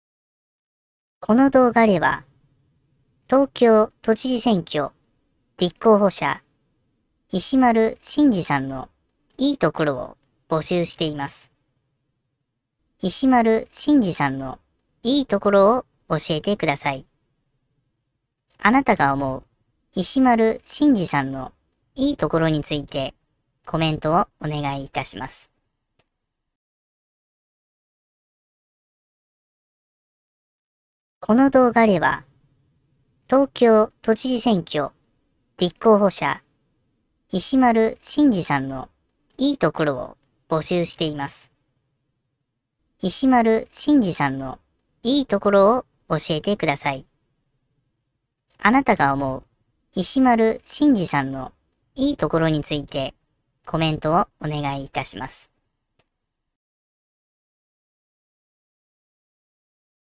石丸伸二さんの良いところを教えて！
石丸伸二さんのポジティブな部分に注目したいと思います。どんどんコメントしてください。